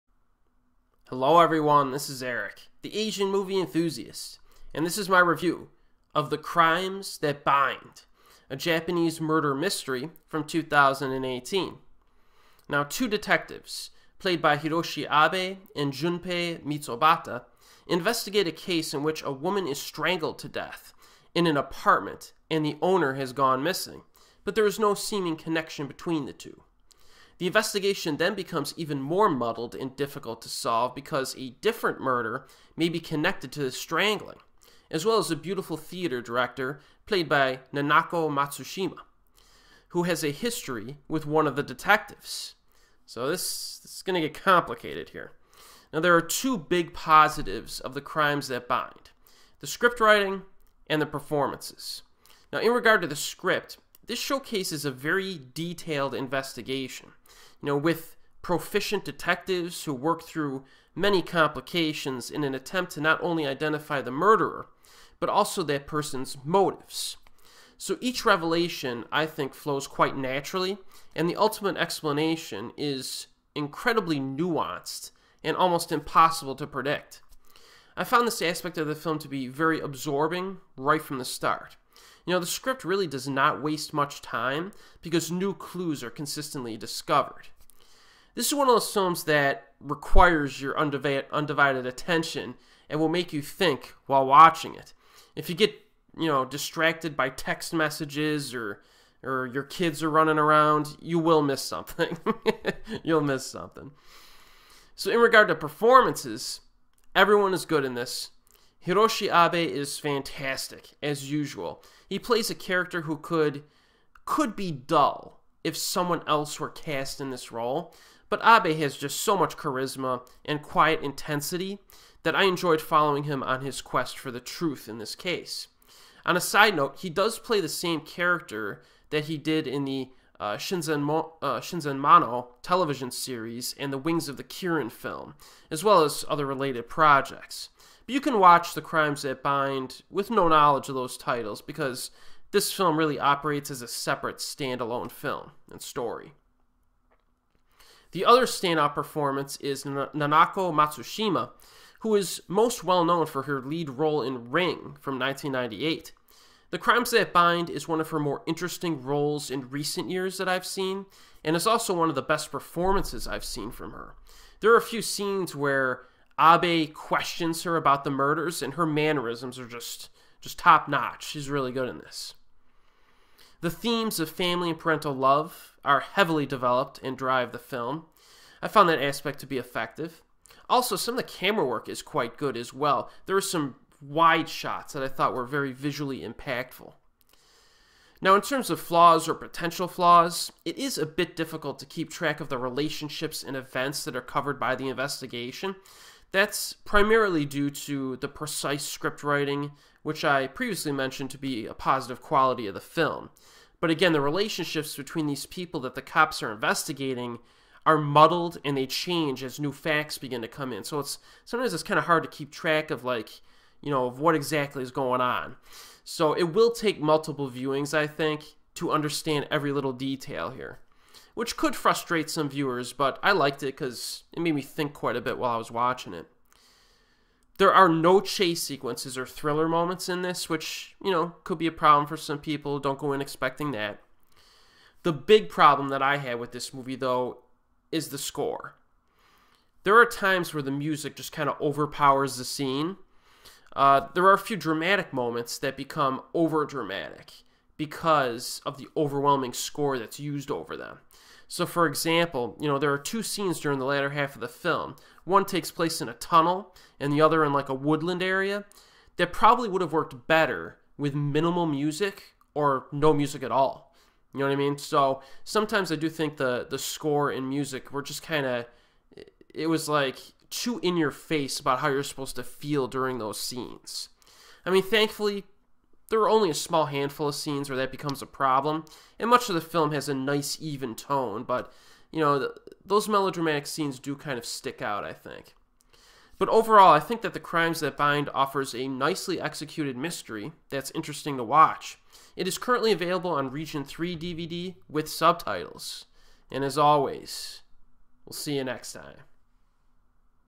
The Crimes That Bind (2018) - Japanese Movie Review
Erik, the Asian Movie Enthusiast presents:

A review of “The Crimes That Bind”, a Japanese murder mystery from 2018 that stars Hiroshi Abe and Nanako Matsushima.

Two detectives (played by Hiroshi Abe and Junpei Mizobata) investigate a case in which a woman is strangled to death in an apartment and the owner has gone missing, but there is no seeming connection.  The investigation then becomes even more muddled and difficult to solve because a different murder may be connected to the strangling, as well as a beautiful theater director (played by Nanako Matsushima) who has a history with one of the detectives.

This is a good mystery flick.